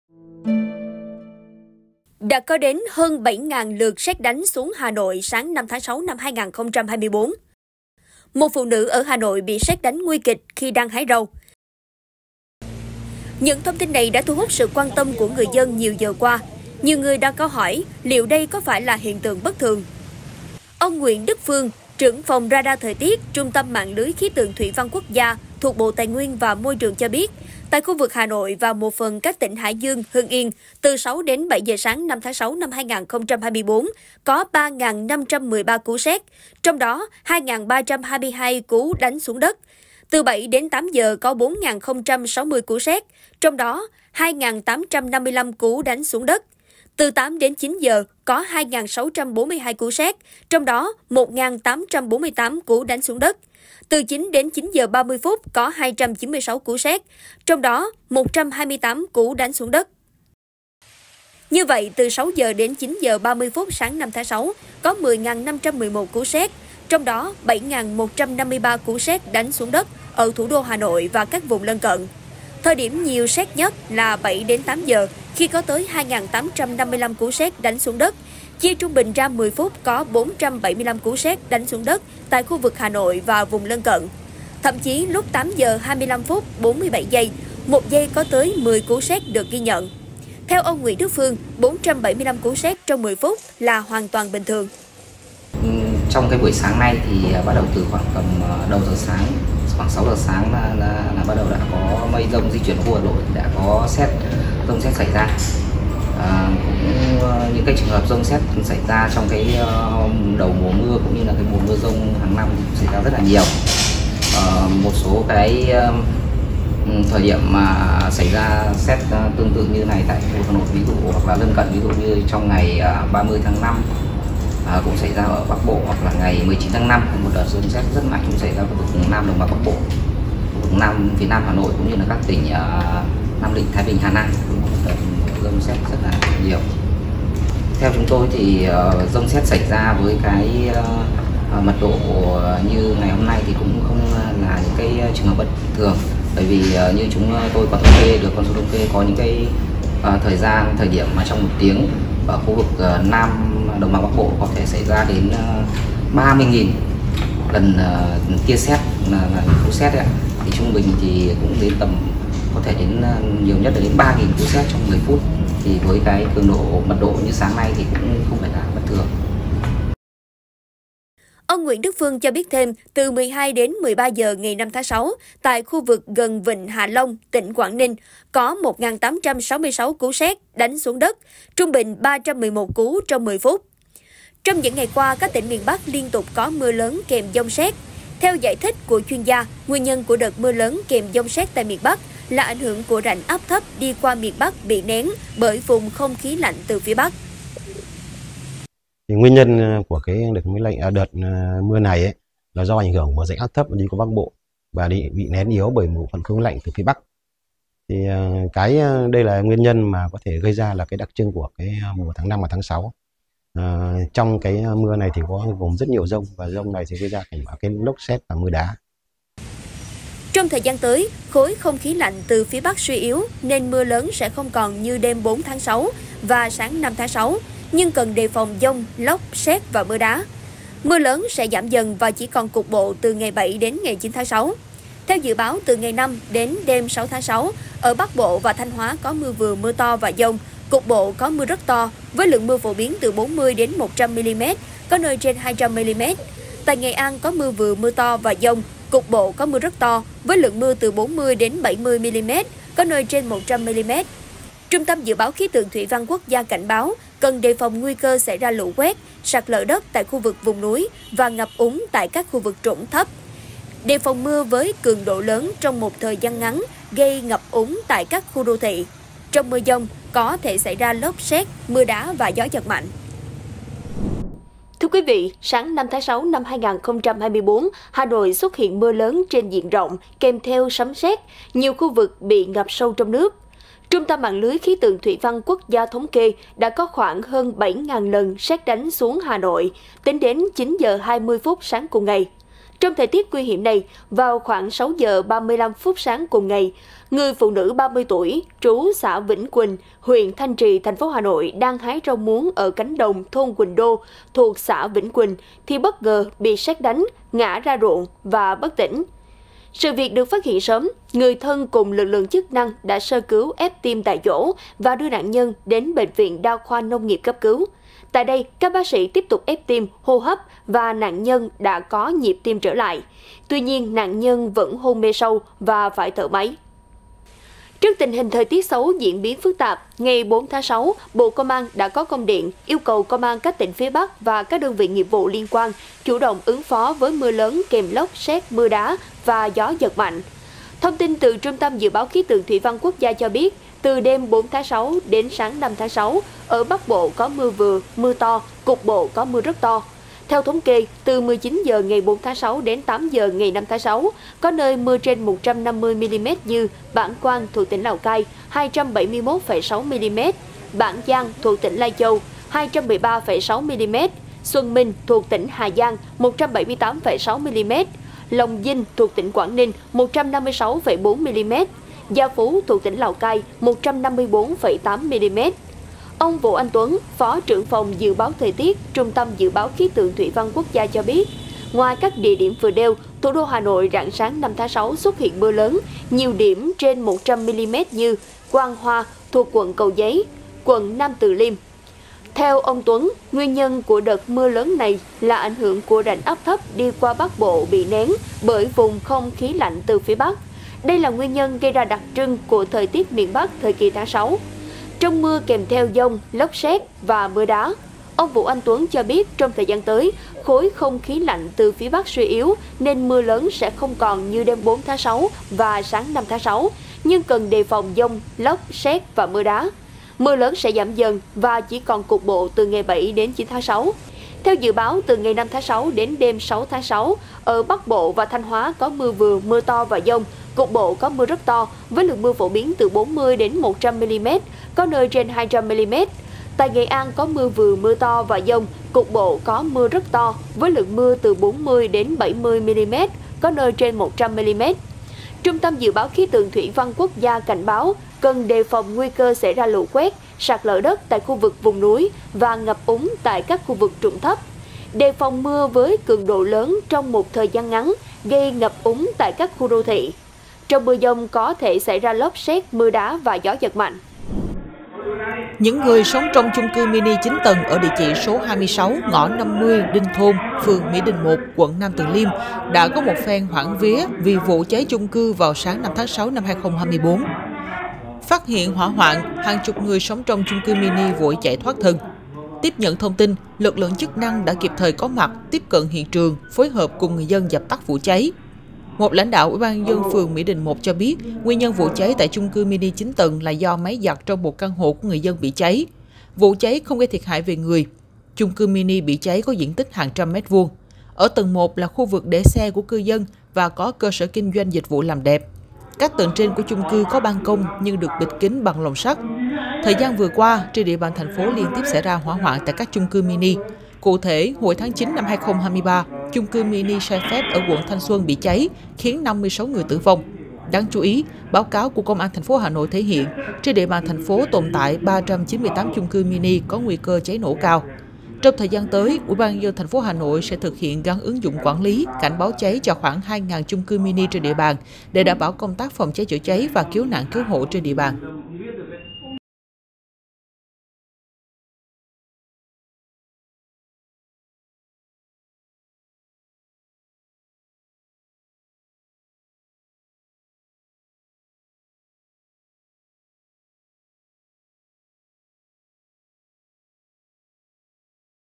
Sét đánh xuống đất 475 lần trong 10 phút ở Hà Nội là "bình thường"
00:00 Sét đánh xuống đất 475 lần trong 10 phút ở Hà Nội là 'bình thường'
04:35 Một phụ nữ ở Hà Nội bị sét đánh nguy kịch khi đang hái rau
07:44 Cháy chung cư mini 9 tầng ở Hà Nội: Hàng chục người chạy thoát thân

Theo ý kiến chuyên gia, sáng 5.6.2024, Hà Nội và các vùng lân cận đã xuất hiện tổng cộng 10.511 cú sét, trong đó 7.153 cú sét đánh xuống đất.

Trong lúc hái rau muống, một phụ nữ ở Hà Nội bị sét đánh trúng ngã ra ruộng. Dù được đưa đi cấp cứu kịp thời, nạn nhân vẫn đang trong tình trạng hôn mê và phải thở máy.

Chung cư mini 9 tầng trên địa bàn quận Nam Từ Liêm (Hà Nội) bất ngờ xảy ra cháy, nhiều cư dân phát hiện vụ việc vội chạy thoát thân.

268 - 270 Nguyễn Đình Chiểu, Phường Võ Thị Sáu, Q.3, TP HCM